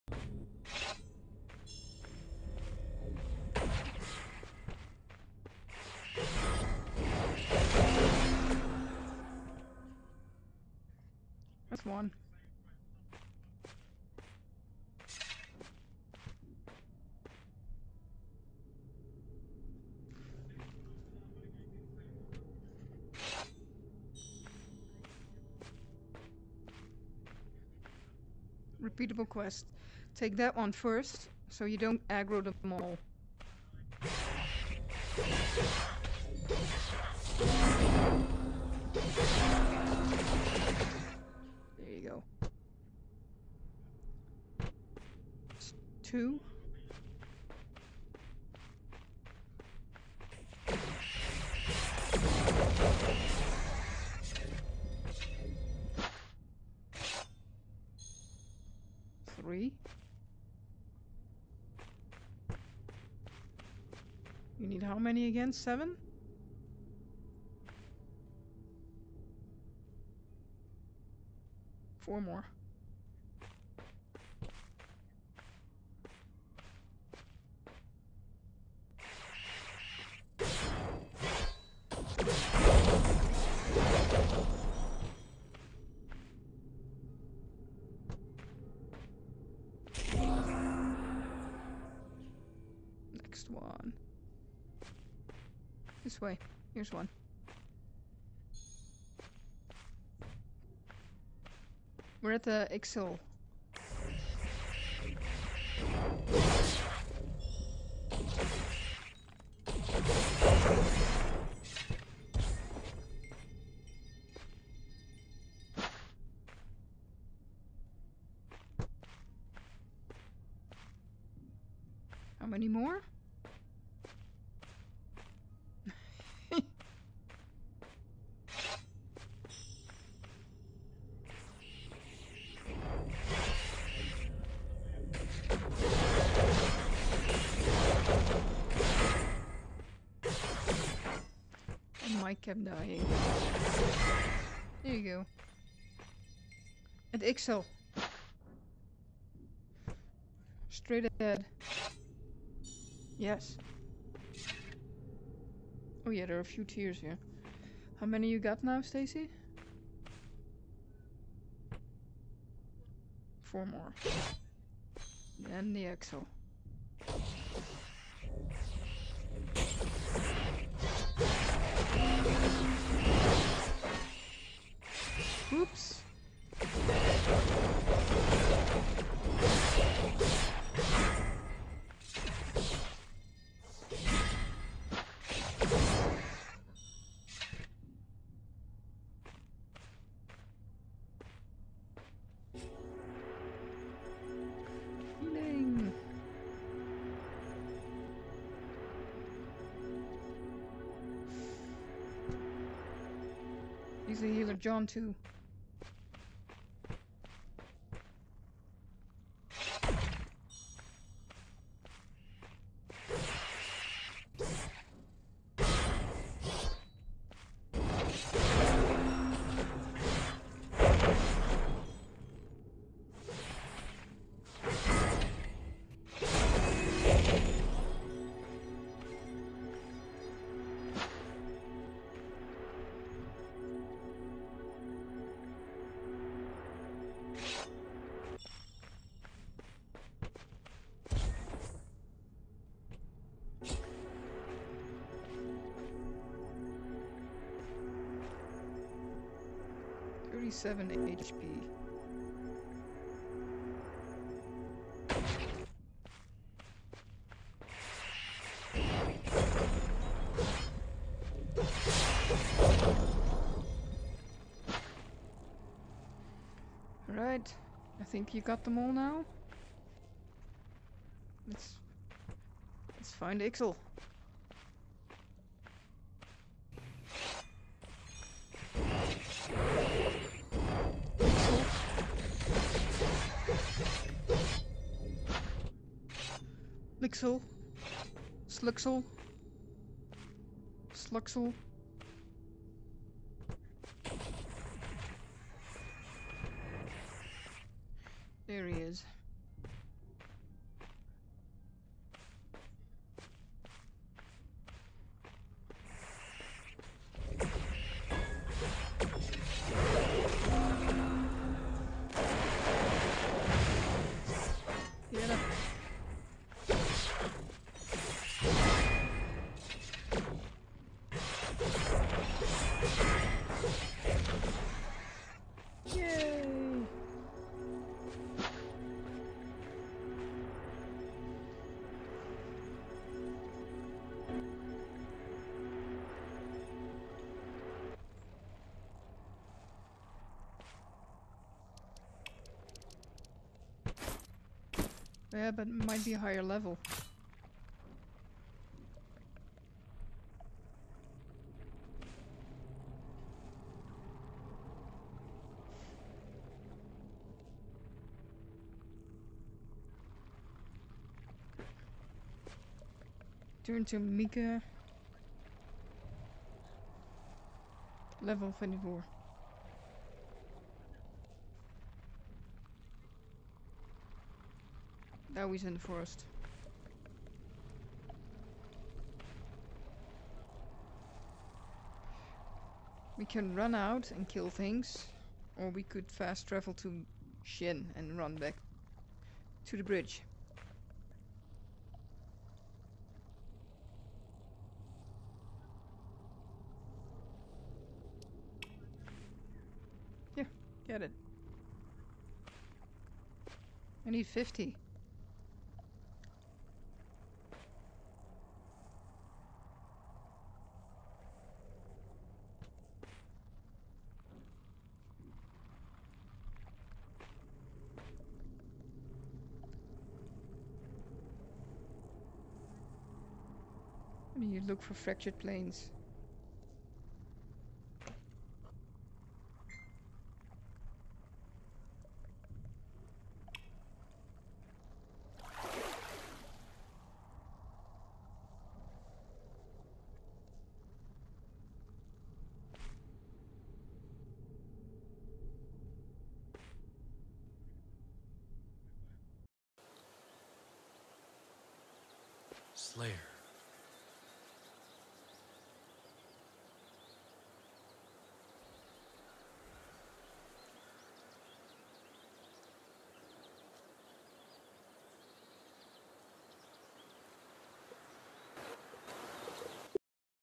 Taking on Lxxl Ixxl with the IDU group.
Taking on the first group boss with the IDU Guild.